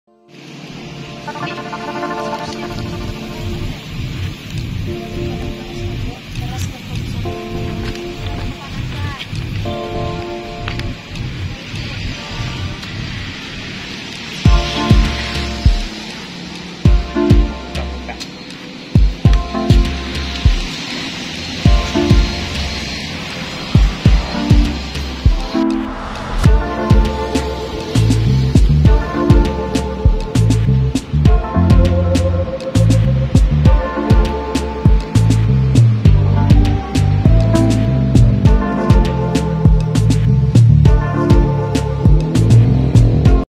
Menikmati suasana jajanan pinggiran jalan sekitar Jalan Ir. PHM Noor Tabalong Kalimantan Selatan.
Sekitar pinggir an Jalan di mulai dari sore hari banyak di temui penjual makanan dan minuman.

Suasana musim penghujan, bakaran salah satu yang diminati, sebagai penghangat di saat dingin.